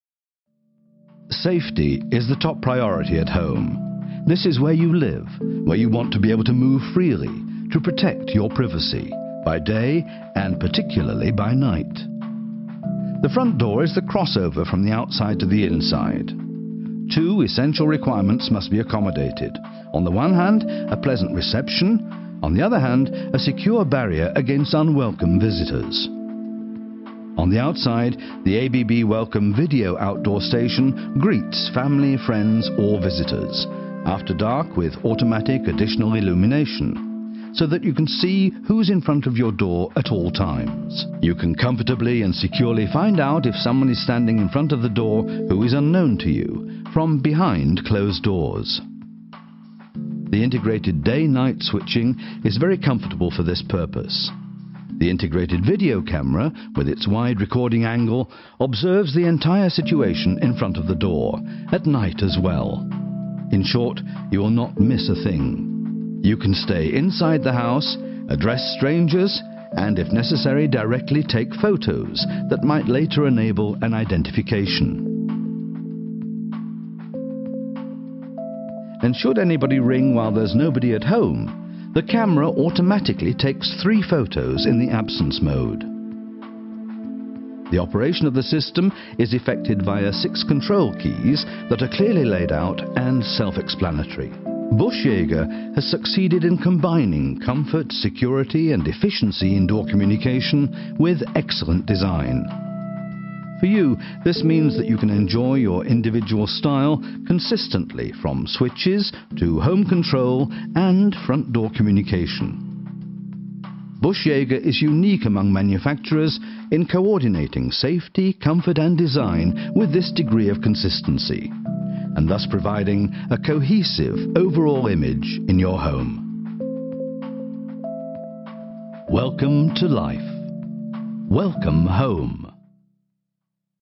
BUSCH-JAEGER international - ABB-Welcome
ABB-Welcome outdoor video station

All ABB-Welcome outdoor stations are available in 2 material versions and many bell module variations. They are suitable for all types of facades and applications. For one front door or several apartments. The ABB-Welcome outdoor stations are optionally fitted with 1, 2, or 3 key modules. It makes them ideal for a house with 1 to 3 families. For houses with several apartments there is the ABB-Welcome outdoor audio station with 4, 6, or 15 bells. The outdoor video station is available with 4, 6, 8 or 12 bells. The number of bells can be grouped according to requirement by combining the module versions. All versions are technically identical.